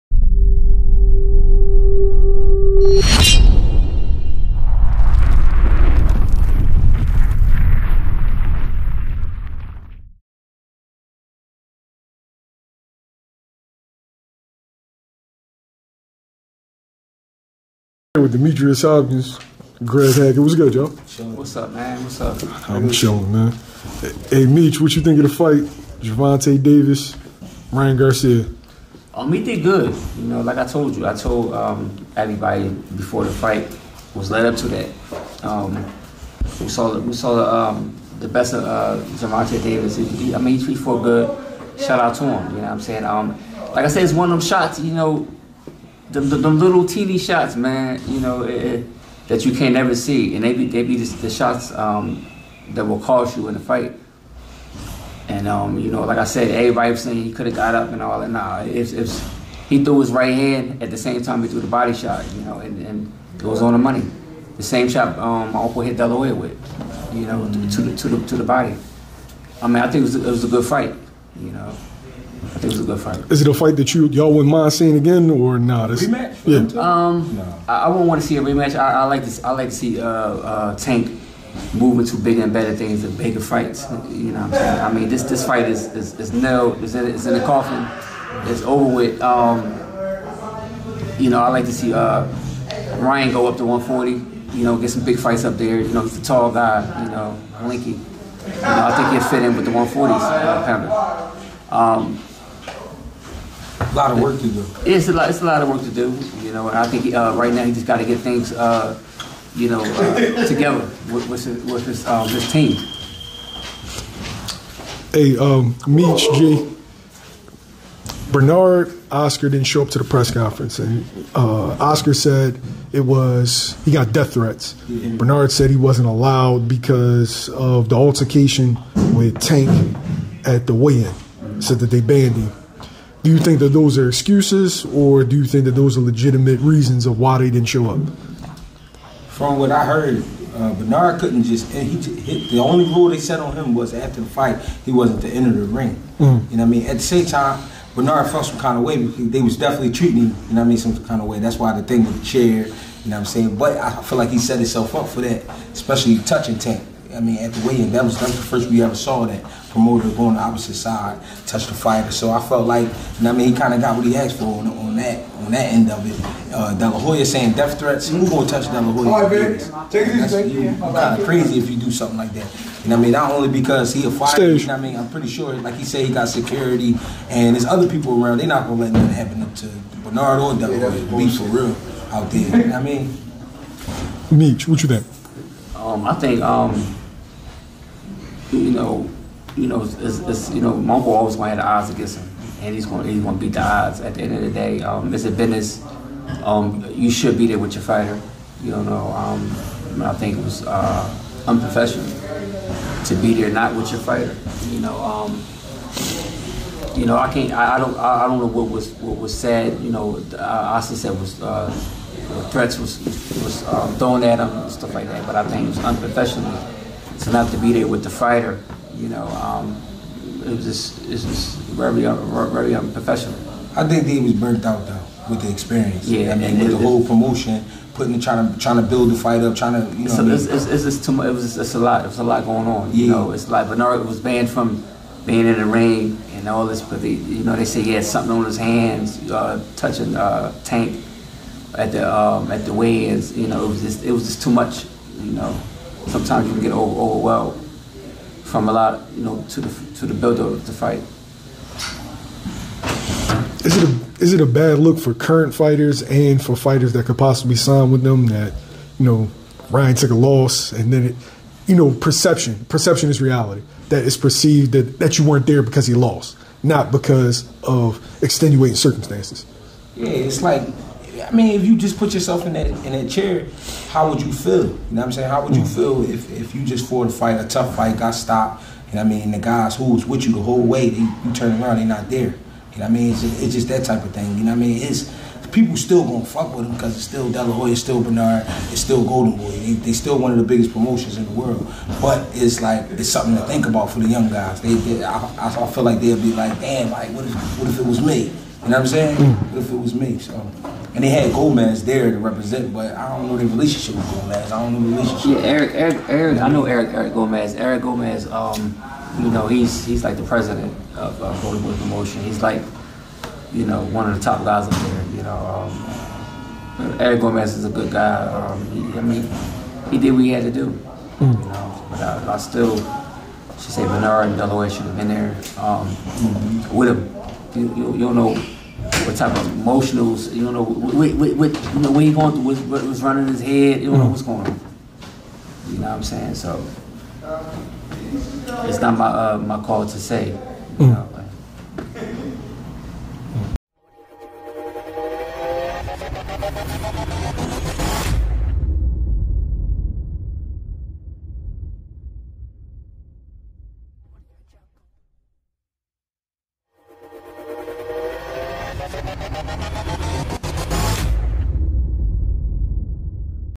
Demetrius Hopkins & Greg Hackett reacts to Gervonta Davis 7th Round win over Ryan Garcia
Veteran Junior Middleweight Contender Demetrius Hopkins and Trainer Greg Hackett gives their thoughts on Gervonta Davis 7th Round Bodyshot Knockout of Ryan Garcia in last Saturdays bout in Las Vegas Nevada.